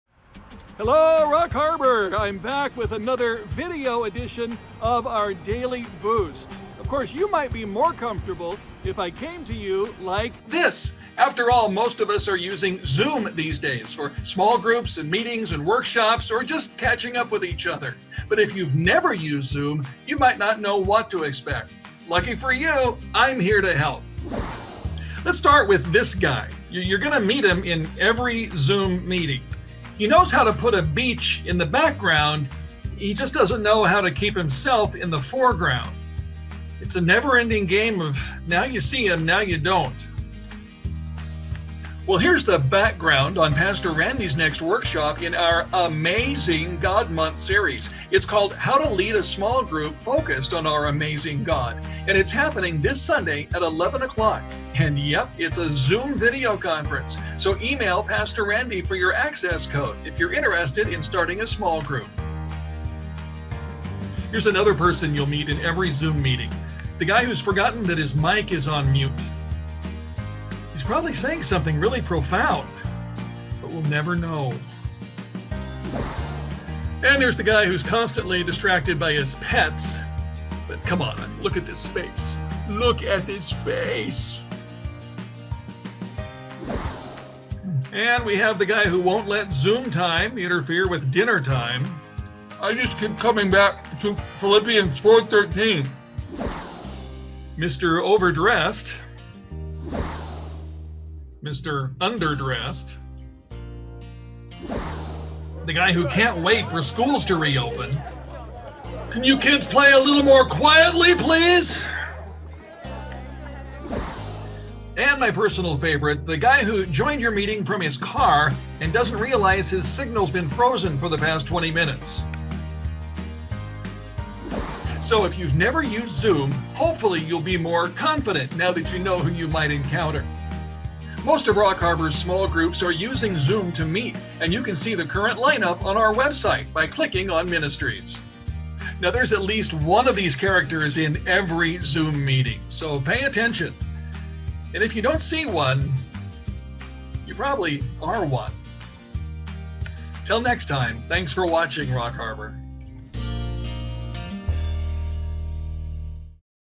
RHCF Daily Boost 5 21 20
All kinds of people are using Zoom during the quarantine.
Here's a look at just a few of them...